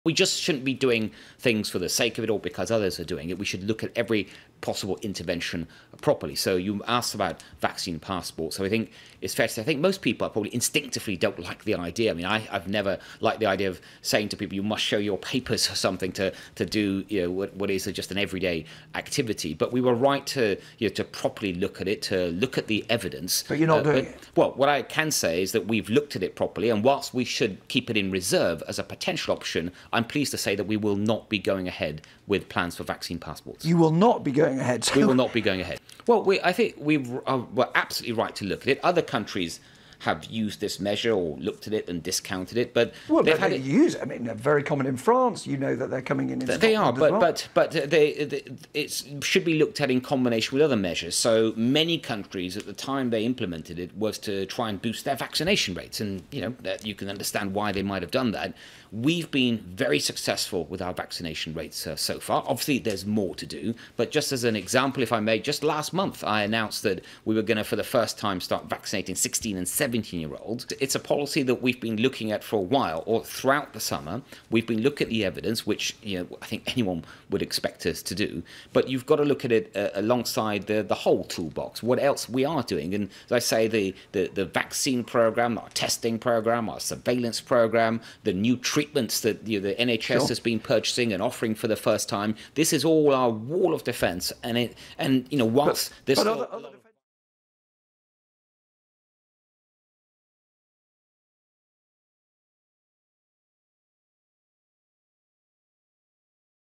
Government scraps plans to introduce vaccine passports for crowded events
PLANS to introduce Covid vaccine passports have been SCRAPPED as Sajid Javid confirms people won't need them to get into nightclubs and cinemas.

The Health Secretary revealed this morning that Covid jab passports won't be introduced as a measure to fight the virus as we enter the winter months.

No more ‘irresponsible’ Covid lockdowns and Brits WILL get Christmas this year, Sajid Javid vows

PCR tests for double-vaccinated Brit holidaymakers will be SCRAPPED ‘as soon as possible’, Sajid Javid confirms